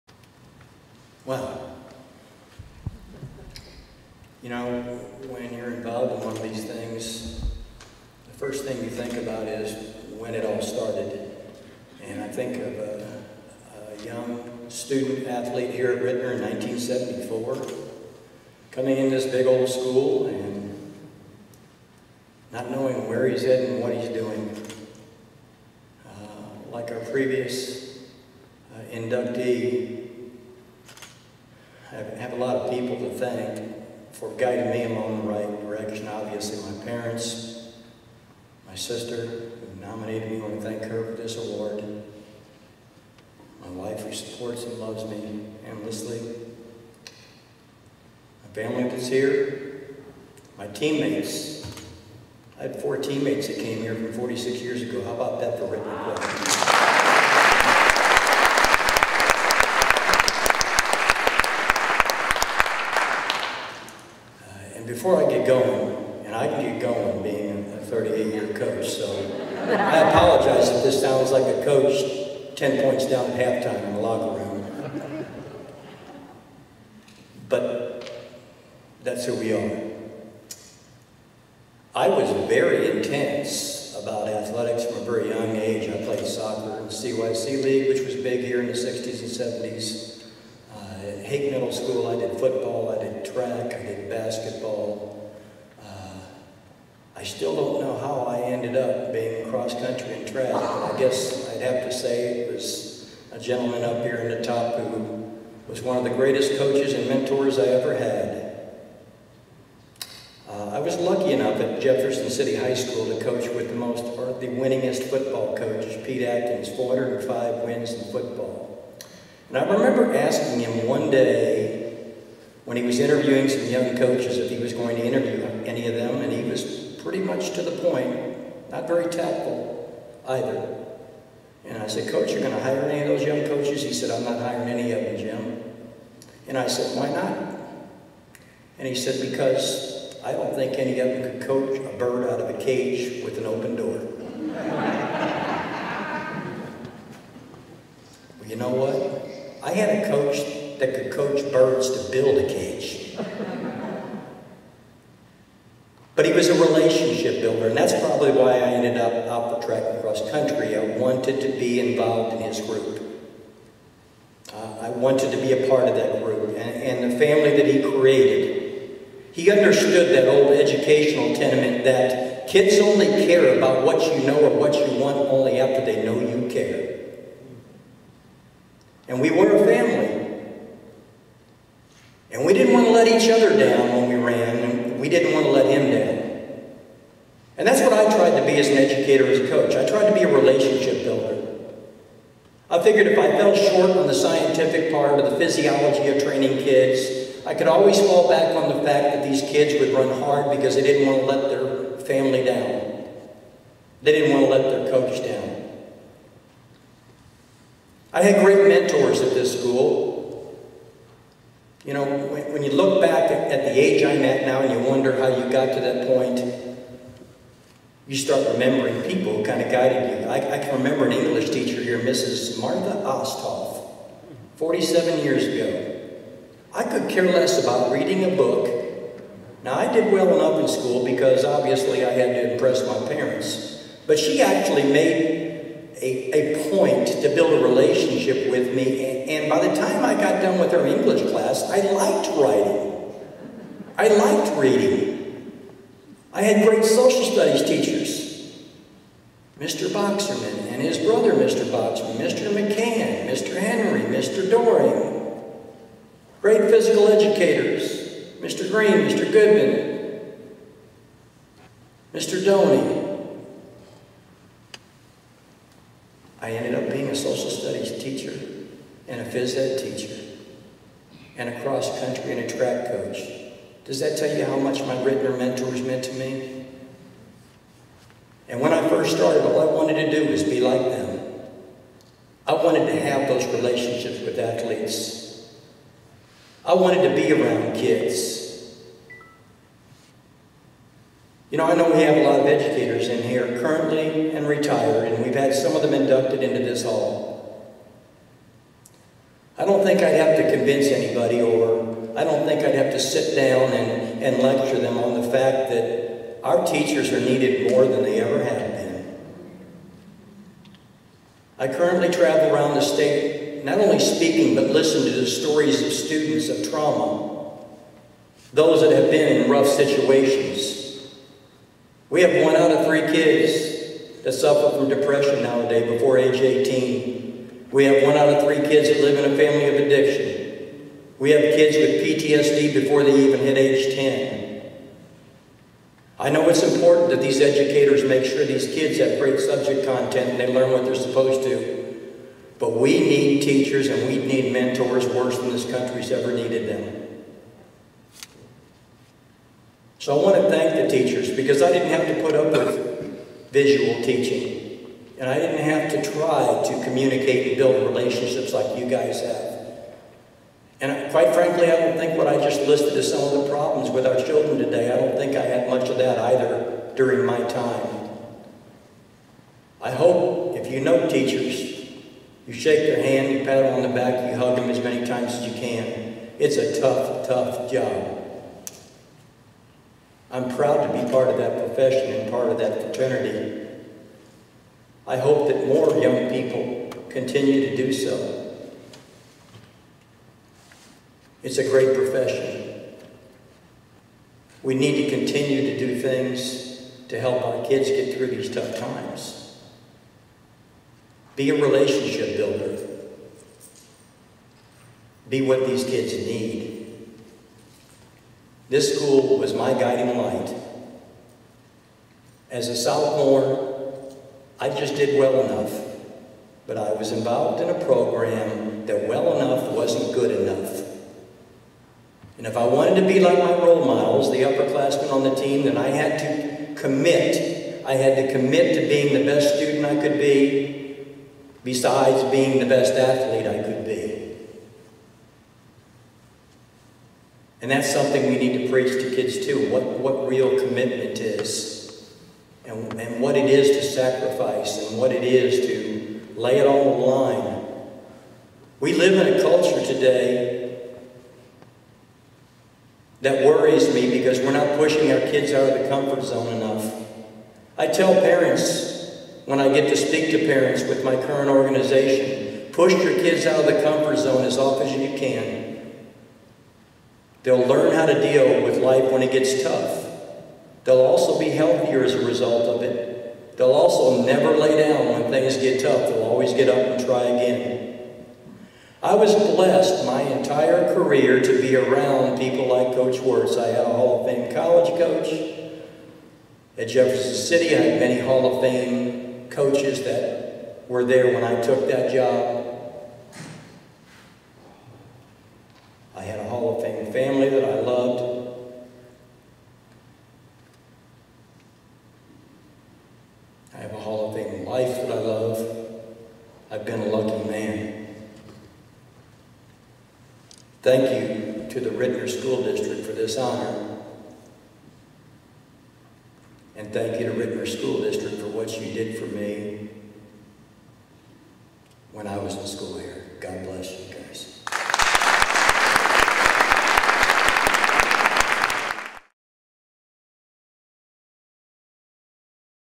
Ritenour Hall of Fame - Jim Marshall Induction Speech (2021)
Jim Marshall is an award-winning social studies and physical education teacher, coach and public speaker with a 38-year educational career. He is the founder of Cody’s Gift, a not-for-profit substance use prevention and coping skills education program for schools. This program was named after his son.

After a successful athletic and academic career at Ritenour High School, he graduated in 1976. He went on to earn his bachelor’s in physical education, health and social studies. He earned a master’s equivalency through Jefferson City School District and Lincoln University in 1988. He was a teacher and four-sport coach at Kickapoo High School and North Callaway Schools from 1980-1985, before landing at Jefferson City High School, where he would teach and coach for the next 25 years.

Jim Marshall led the Jefferson City cross country  boys and girls program to 34 district titles, 16 top four finishes in the state, and six runner-up or state championships. While he was the head assistant coach in track, Jefferson City won nine state titles and 24 top four finishes, with 43 district titles. He was named Missouri Coach of the Year three times and was inducted into the Missouri Track and Cross Country Coaches Hall of Fame in 2007. The Jefferson City track program was inducted into the Missouri Sports Hall of Fame as an outstanding high school program in 2013.

Jim retired in 2010 from public schools and spent the next eight years as a track coach at Westminster College and William Woods University. He won several conference titles, was named Coach of the Year twice, and coached five All-Americans. He retired in 2018.

During the last 10 years, Jim has spoken about substance use prevention at 700 schools, conferences, workshops and seminars in all 52 counties in Missouri, as well as out of state. His mantra on substance use talks is, “If we can prevent or save just one...” He was awarded the Drug Free Youth Educator of the Year Award in 2014 by Major League Baseball.

Jim has lobbied, testified and worked on legislation relating to substance use prevention. In 2018, Bailey and Cody’s Law was passed in Missouri. The Good Samaritan Law prevents prosecution of people that bring an overdosing person into a hospital. In 2021, the Prescription Drug Monitoring Act was passed after 10 years of repeated attempts. Missouri was the only state that did not have this program. It prevents prescription and doctor shopping and gives physicians an adequate data base to help them prescribe safely.

Friends and colleagues praise Jim’s modesty, integrity and reliability. He has been a role model to thousands of young people. He is blessed with a wonderful wife, Cheryl, and loves keeping up with his daughter, Brittanny, along with his three grandchildren and five nieces. Because of his accomplishments and community service, we are honored to welcome him into our Hall of Fame.